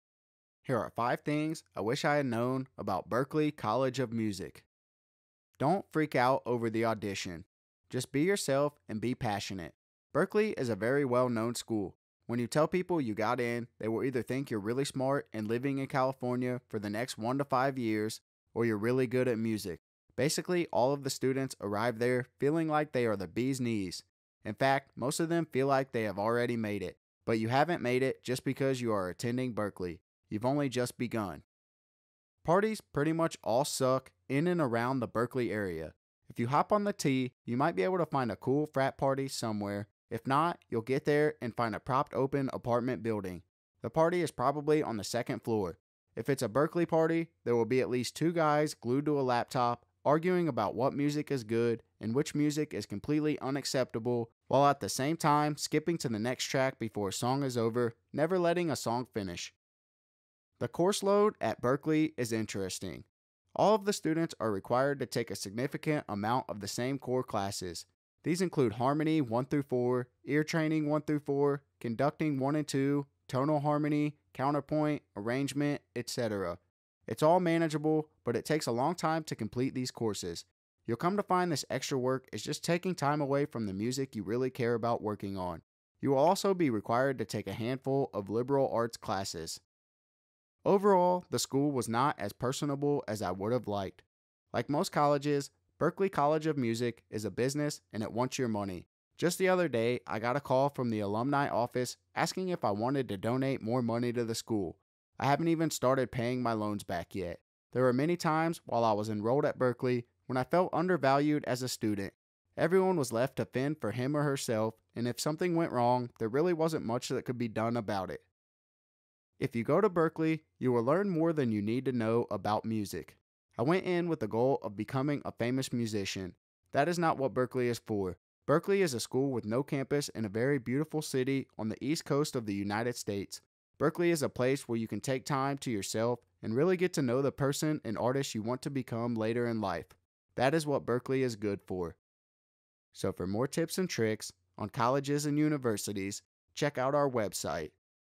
Berklee College of Music - 5 Things I Wish I Had Known Before Attending
- Berklee College of Music is the most highly respected contemporary music school in the country. Contemporary meaning jazz. I completed eight semesters there in three years with a major in Electronic Production and Design. Two of those semesters were taken during hot and humid summers that were slightly cheaper, shorter, and less busy.